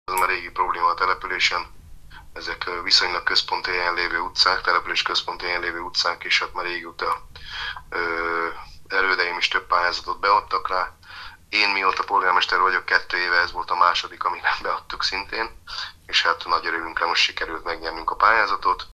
Útfelújítás Faddon
A Magyar Falu Program pályázatán 35 millió Forint vissza nem térítendő támogatást nyert Fadd nagyközség önkormányzata a Béke-köz és az Arany-köz útfelújítására. Számolt be a hírről Bordács József, a település polgármestere Facebook oldalán. A munkálatok várhatóan jövő tavasszal kezdődnek.